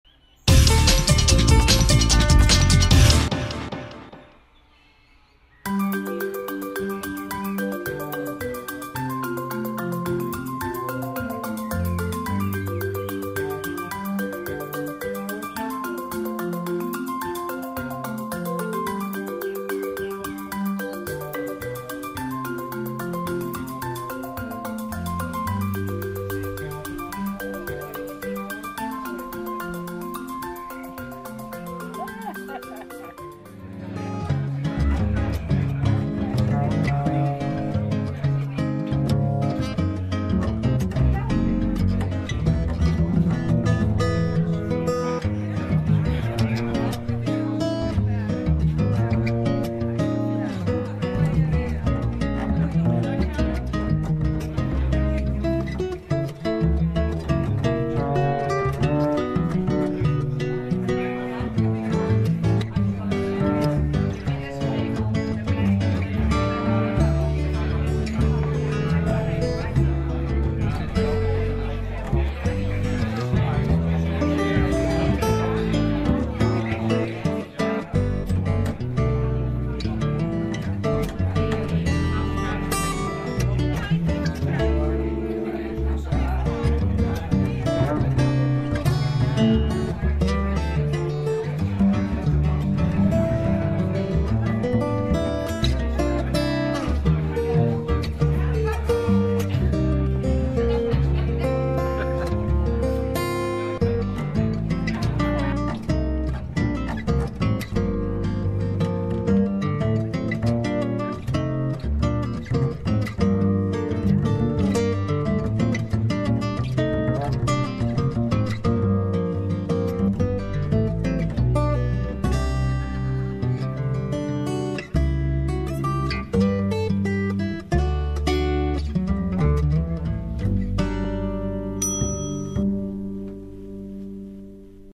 Expositie in theetuin de Bongerd, Herkingen.
Korte openingsimpressie van een expositie van Josina van Prooijen in Theetuin de Bongerd, Herkingen.